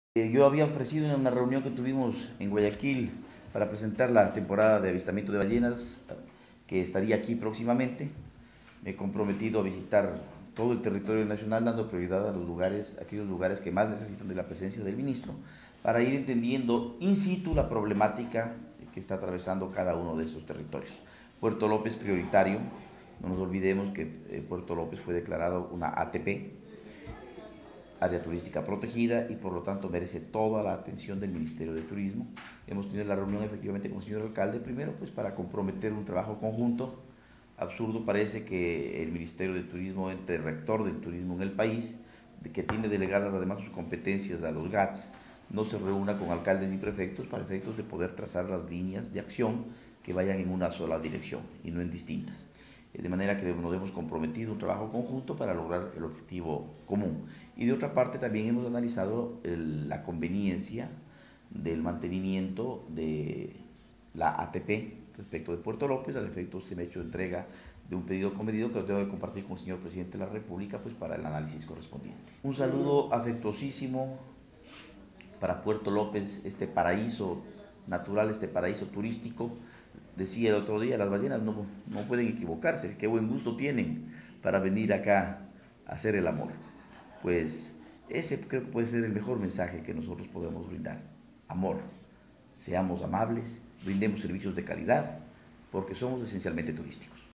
ENRIQUE PONCE DE LEÓN - MINISTRO TURISMO
ALCALDE SUBROGANTE SE REUNE CON MINISTRO DE TURISMO  PARA CONOCER TRABAJO  EN EL CANTON